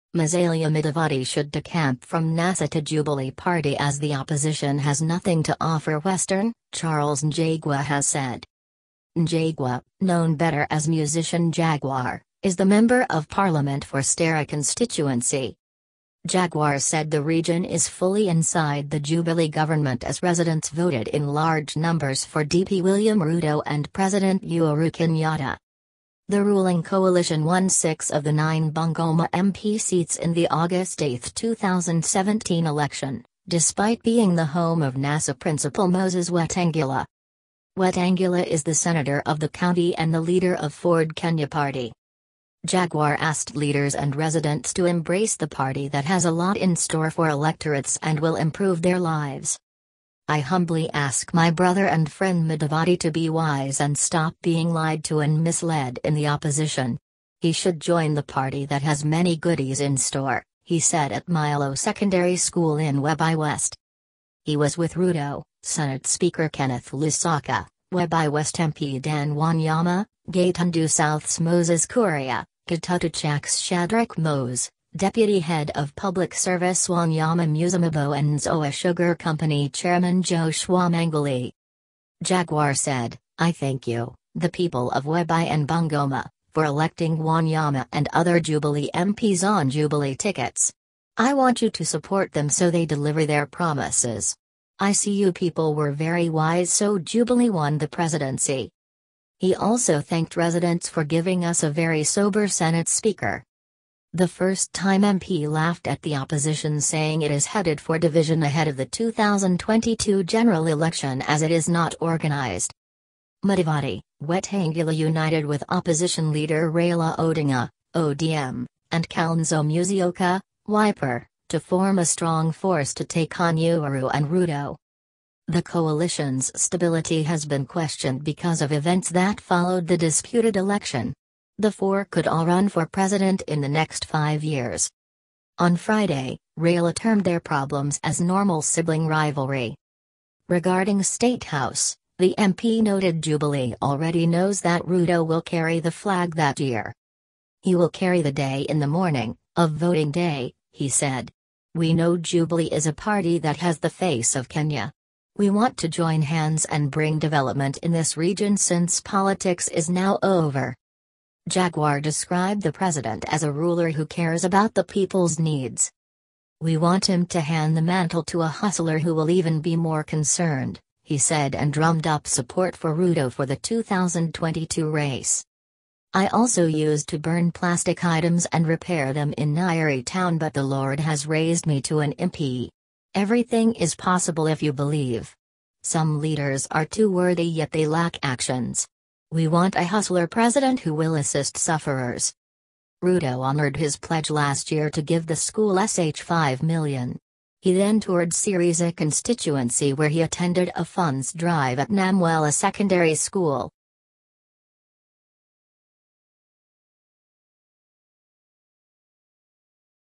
Dump Raila for Uhuru, Nasa going down, Jaguar tells Mudavadi
Musalia Mudavadi should decamp from Nasa to Jubilee Party as the Opposition has nothing to offer Western, Charles Njagua has said.

Njagua, known better as musician Jaguar, is the Member of Parliament for Starehe constituency.

Jaguar said the region is "fully inside the Jubilee government" as residents voted in large numbers for DP William Ruto and President Uhuru Kenyatta.

The ruling coalition won six of the nine Bungoma MP seats in the August 8, 2017 election, despite being the home of Nasa principal Moses Wetang'ula.

Wetang'ula is the Senator of the county and the leader of Ford Kenya party.

Jaguar asked leaders and residents to embrace the party that has a lot in store for electorates and will improve their lives.

"I humbly ask my brother and friend Mudavadi to be wise and stop being lied to and misled in the Opposition. He should join the party that has many goodies in store," he said at Milo Secondary School in Webuye West.

He was with Ruto, Senate Speaker Kenneth Lusaka, Webuye West MP Dan Wanyama, Gatundu South's Moses Kuria, Kitutu Chache's Shadrack Mose, Deputy Head of Public Service Wanyama Musimabo and Nzoia Sugar Company chairman Joash Wamangoli.

 Jaguar said: "I thank you, the people of Webuye and Bungoma, for electing Wanyama and other Jubilee MPs on Jubilee tickets. I want you to support them so they deliver their promises. I see you people were very wise so Jubilee won the presidency."

He also thanked residents for "giving us a very sober Senate Speaker".

The first-time MP laughed at the Opposition saying it is headed for division ahead of the 2022 general election as it is not organised.

Mudavadi, Wetang'ula united with Opposition leader Raila Odinga (ODM) and Kalonzo Musyoka (Wiper) to form a strong force to take on Uhuru and Ruto.

The coalition's stability has been questioned because of events that followed the disputed election. The four could all run for president in the next five years.

On Friday, Raila termed their problems as normal sibling rivalry.

 Regarding State House, the MP noted Jubilee already knows that Ruto will carry the flag that year.

"He will carry the day in the morning [of voting day]," he said. "We know Jubilee is a party that has the face of Kenya. We want to join hands and bring development in this region since politics is now over."

Jaguar described the President as a ruler who cares about the people's needs.

"We want him to hand the mantle to a hustler who will even be more concerned," he said and drummed up support for Ruto for the 2022 race.

"I also used to burn plastic items and repair them in Nyeri town but the Lord has raised me to an MP. Everything is possible if you believe. Some leaders are too wordy yet they lack actions. We want a hustler President who will assist sufferers."

Ruto honoured his pledge last year to give the school Sh5 million. He then toured Sirisia constituency where he attended a funds drive at Namwela Secondary School.